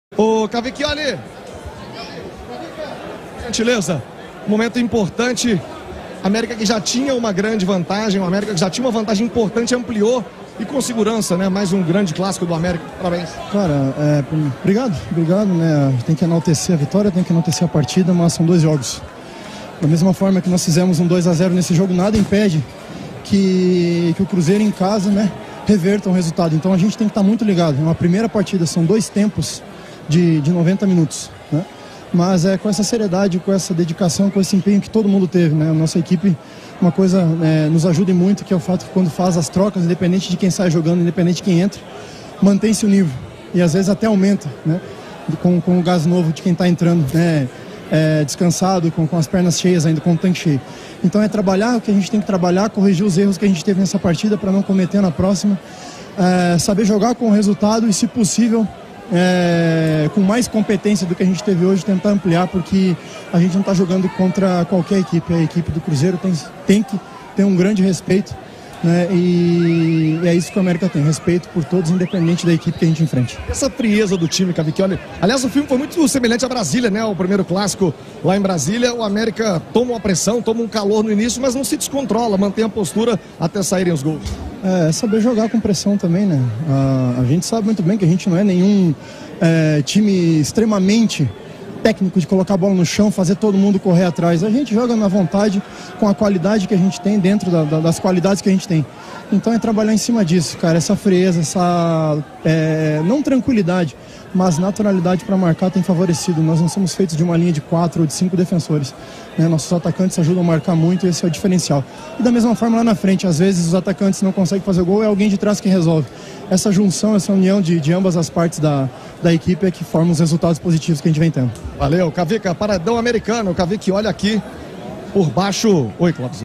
CAVICHIOLI SOBRE VITÓRIA DO AMÉRICA: 'TEM QUE TER UM GRANDE RESPEITO COM O CRUZEIRO'
O goleiro Matheus Cavichioli destacou o desempenho do time do América depois da vitória por 2 a 0 em cima do Cruzeiro. Confira!